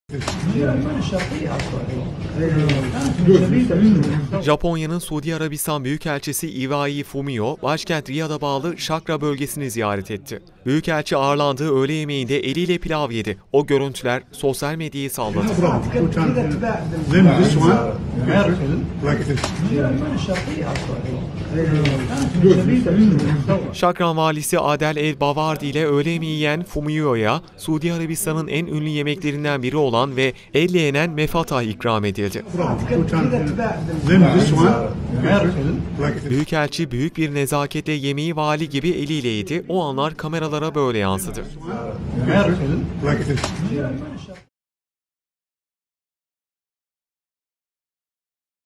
Japonya Büyükelçisi eliyle pilav yedi
Japonya'nın Suudi Arabistan Büyükelçisi Iwai Fumio, Şakra bölgesini ziyareti sırasında Vali Adel Al-Bawardi ile birlikte öğle yemeği yedi.  Büyükelçiye, Arabistan'ın ünlü yemeklerinden olan ve elle yenmesi gereken Mfatah ikram edildi.

Mfatah'ı elle yiyebilmek için Vali Adel Al-Bawardi'nin tavsiyelerini dinledim." ifadelerini kullandı.

TÜRKSAT 4A
Frekans: 12729
Sembol: 30000
Polarizasyon: V (Dikey)